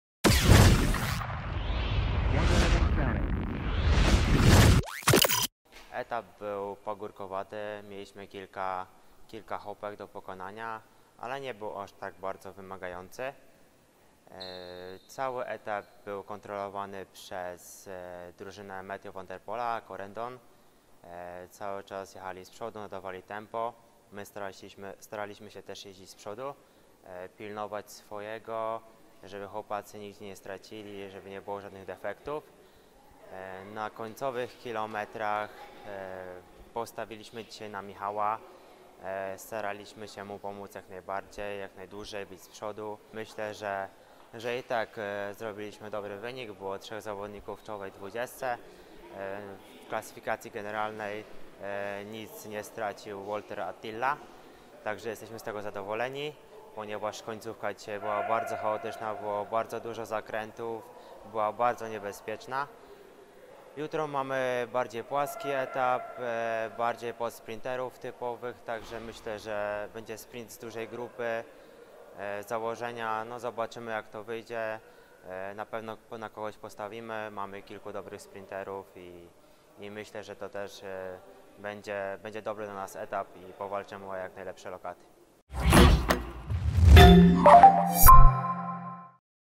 Kamil Małecki podsumowuje pierwszy etap Tour of Antalya | OnetNews
Na pierwszym etapie Tour of Antalya (2.2) kolarze mieli do pokonania 143 km. Trasa była pagórkowata, zawodnicy zmagali się z wąskimi drogami. Sporą część etapu stanowił odcinek, który można było określić mianem szutrowego. Najwyżej z „Pomarańczowych” sklasyfikowany był Michał Paluta, który zakończył etap na 8. miejscu, 11. był Attila Valter, a 16. Patryk Stosz. Wygrał Mathieu van de Poel (Corendon-Circus). W klasyfikacji generalnej Michał Paluta zajmuje 9., a Attila Valter 12. miejsce. Strata każdego z nich do lidera wynosi 10 sekund.